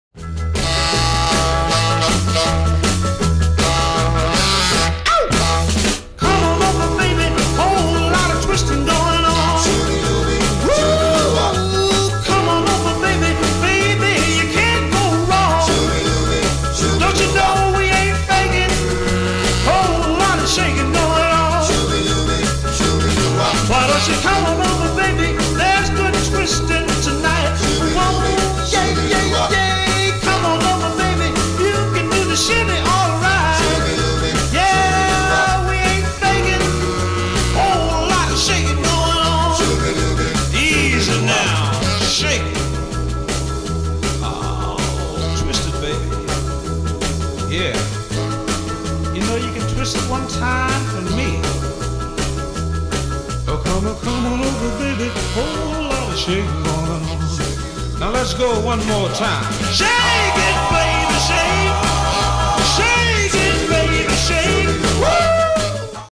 Dubai World Trade Center Rock n Roll Show / PLANET ENTERTAINMENT
Choreography: Yeya Ekstrom
Dancers: Yeya Ekstrom
Emma West
Giselle Rarinca
Karim "Kriman" Benbella